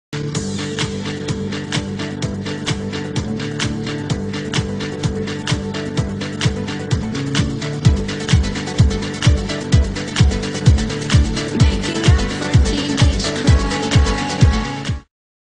intro for my series
that's the second or third intro i made. the other ones, didn't fit with the name of the series. so here it goes..
i will post a trailer too but that will be after i make it xd  cause trailer is moore difficult than the intro.
normally,intros has the names of all the characters. that couldn't happen with mines cause there will be many char.  i still don't know how many & who of them will be the most important. so that's why i just made that kind of intro. anyway.. tell me what u think? but honestly, if u don't like it , tell me.